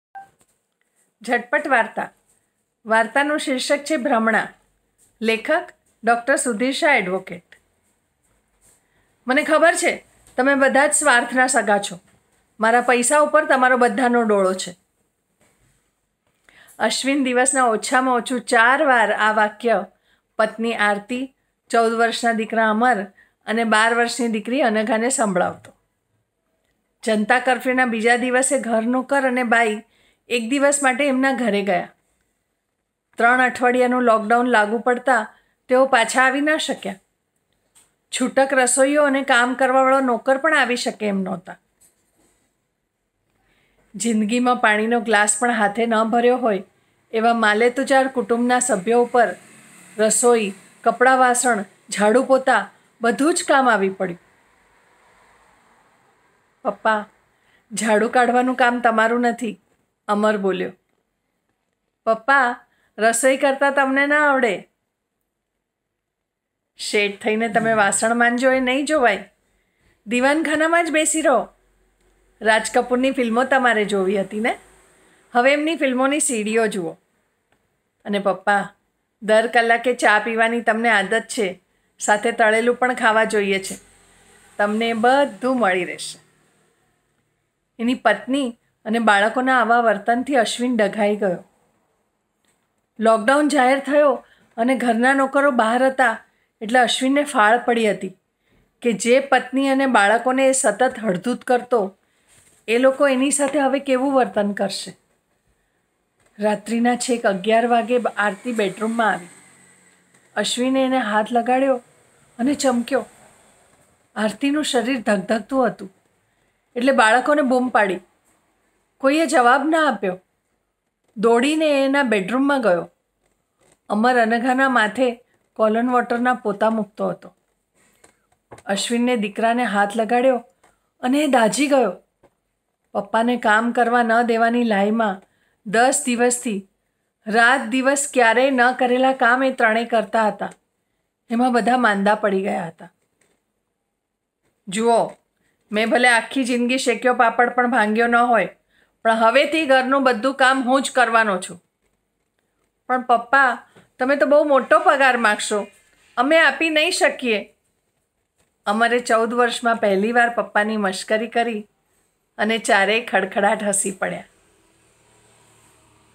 Zatpat Story/ 'Bhramana' / story written by Dr. Sudhir Shah, Advocate
What happens to family relationships due to COVID-19 Lockdown? View today's Zatpat Story 'Bhramana' written by Dr. Sudhir Shah, Advocate and find out. Till lockdown Remain Indoor Remain Safe.